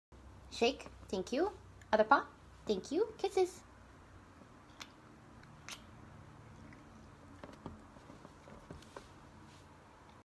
Deb the cat. Shake, other paw, shake, kisses.
My cat is the smartest dumb cat ever. More dog than cat.